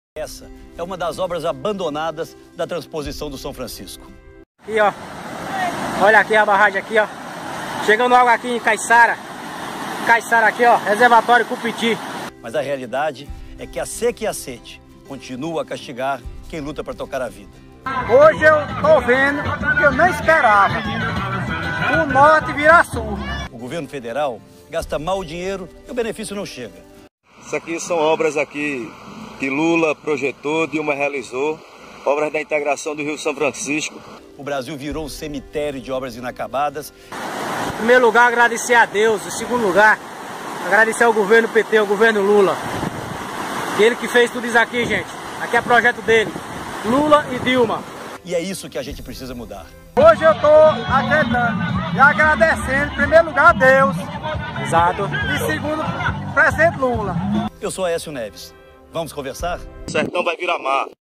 O Aécio Neves, não acreditava na Transposição do Rio São Francisco, Lula e Dilma, fizeram!
O tucano, Aécio Neves e os membros do PSDB, nunca acreditaram na Transposição do Rio São Francisco, mas Lula e Dilma Rousseff, fizeram o sertão virar mar. Hoje, a realização de ter água no Nordeste, foi concretizada, com esta iniciativa do Partido dos Trabalhadores em levar água potável para 12 milhões de nordestinos.